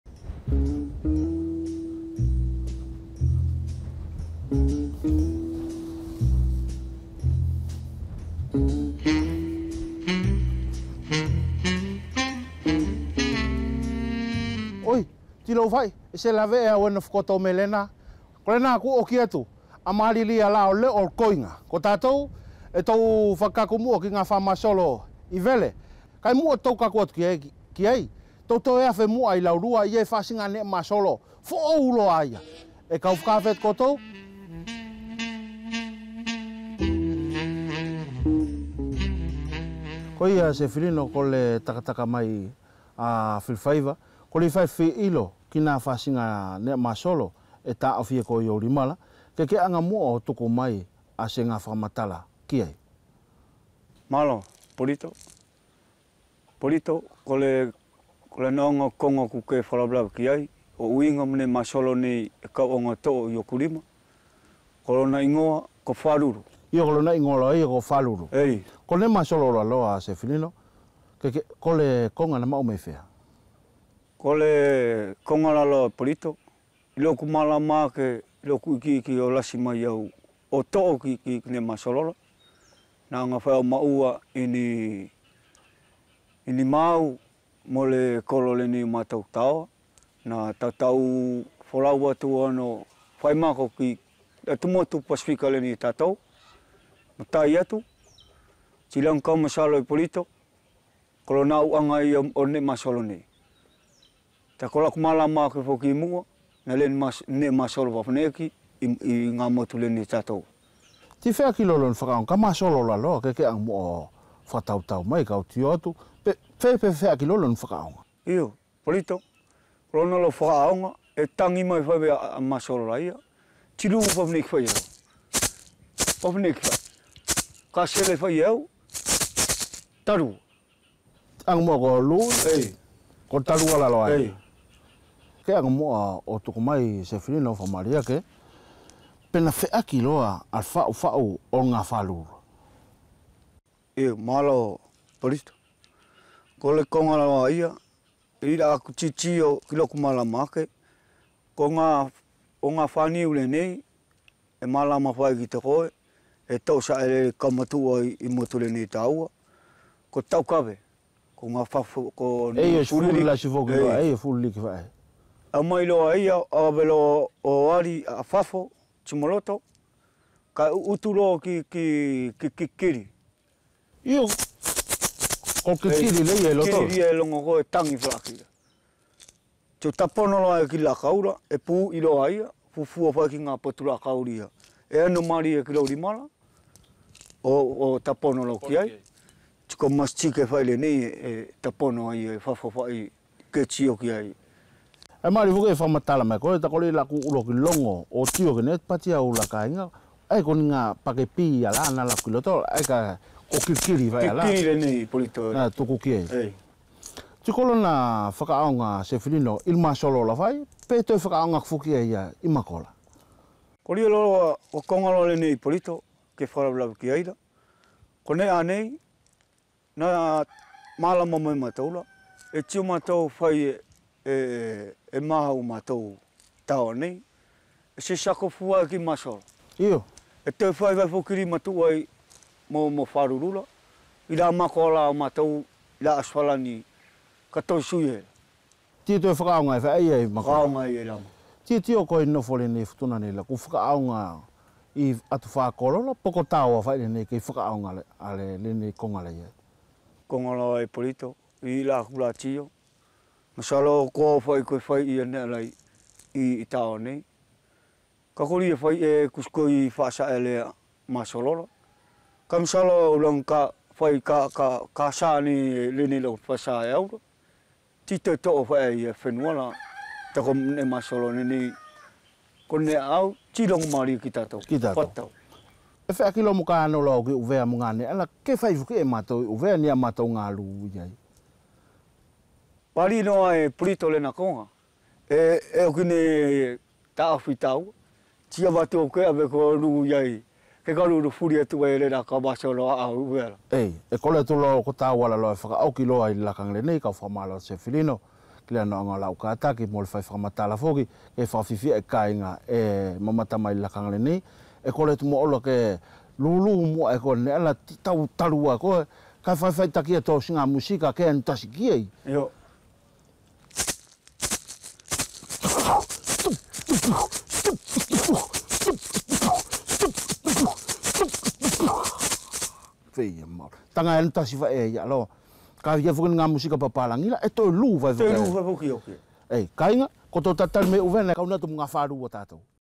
FILIFAIVA: la Panthère rose et Sefilino Masei
Ipolito n'en finit plus de nous surprendre. Le voilà qui campe Pink Panther ! C'est qu'il a plusieurs cordes à son "ukulele" notre Ipolito national. Dans cette séquence hilarante, il nous fait découvrir Sefilino Masei, joueur de Fa lulu, accessoires de musique qui ressemblent aux maracas mexicains.